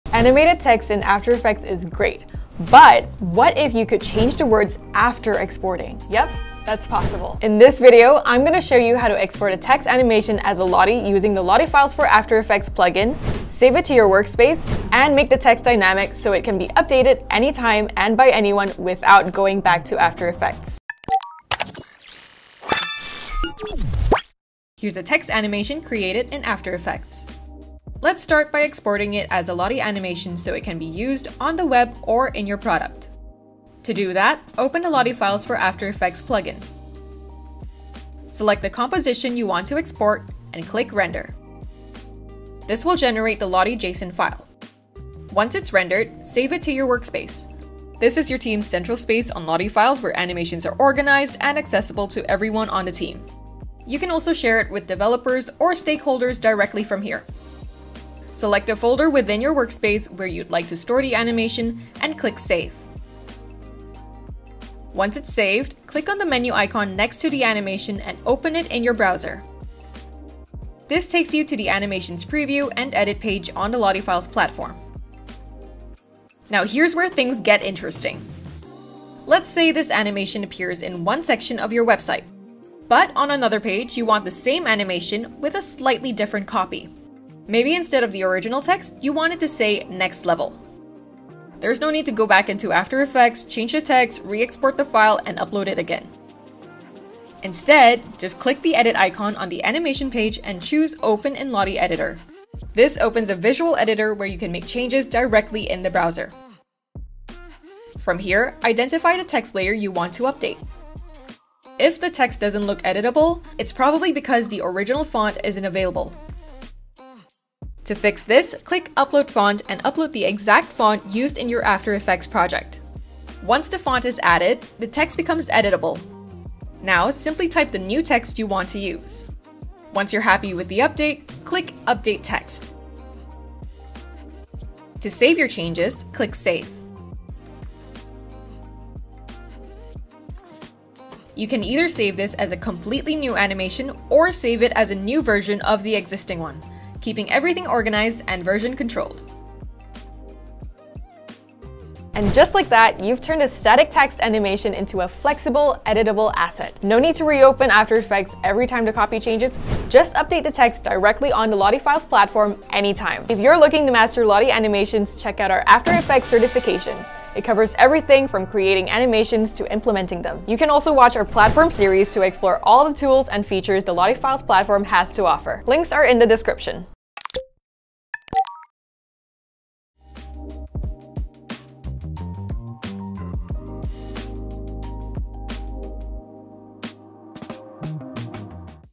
Creating dynamic text animation in After Effects: How to edit text after export
Learn how to export a text animation in After Effects, turn it into dynamic text as a Lottie, and make it editable anytime—by anyone—without going back into After Effects.

Timestamps:

00:25 Exporting text animation as a Lottie
00:45 Saving the Lottie to your workspace
01:06 Loading animation on the LottieFiles platform
01:38 Using Lottie Editor to update text animation
02:20 Saving edited and updated text animation